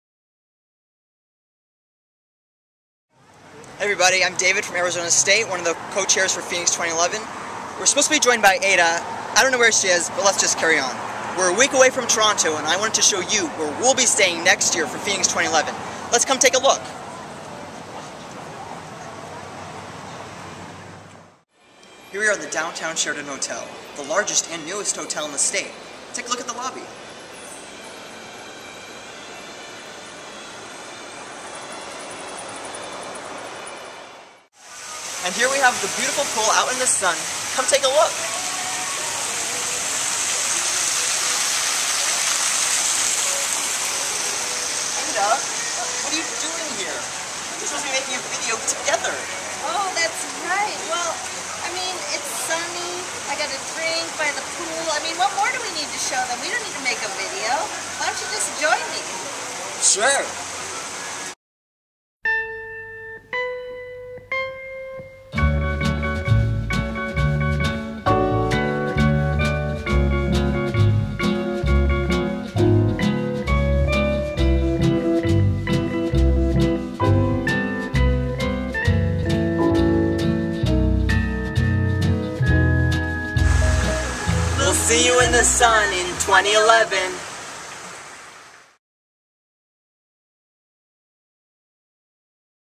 AIAS FORUM 2011 Phoenix Short Hotel Teaser
David and Ada tour the hotel Location for FORUM 2011 Phoenix.